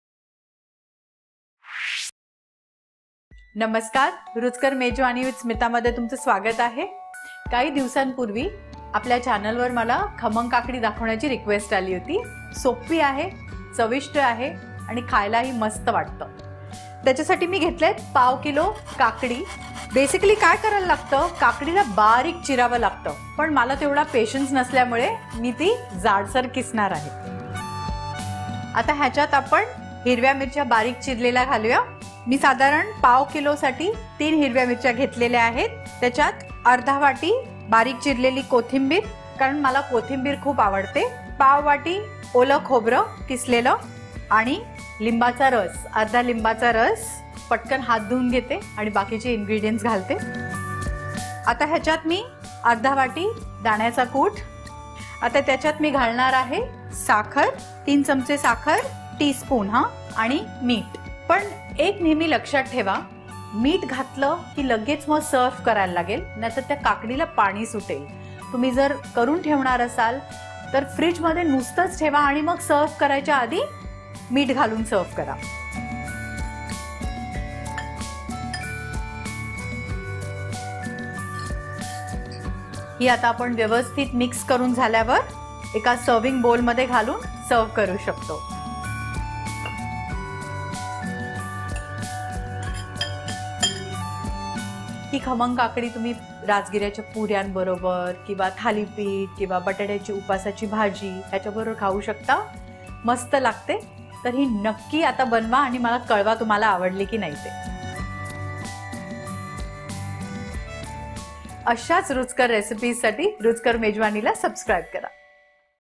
Khamang Kakdi | खमंग काकडी । Cucumber Salad Recipe | Kakdichi Koshimbir | Recipe in Marathi | Smita
Learn How To Make Khamang Kakdi Recipe from Chef Smita Deo only on Ruchkar Mejwani. Cucumber Salad Recipe is a Kakdichi Koshimbir Recipe in Marathi of खमंग काकडी or काकडीची कोशिंबीर. Make this simple and easy Cucumber and Peanut Salad Recipe at your home and share your experience with us in the comments section below.

Ingredients:-
¼ kg. Cucumber peeled and finely chopped or grated thick
½ cup Roasted peanut powder
¼ cup Fresh coconut grated
3-4 Green chillies, finely chopped
Juice of ½ a lemon
2 tsp. Sugar

Method:-
Mix all the ingredients together and serve.

Khamang Kakdi is ready to eat!

HAPPY COOKING!!!

Host: Smita Deo
Copyrights: REPL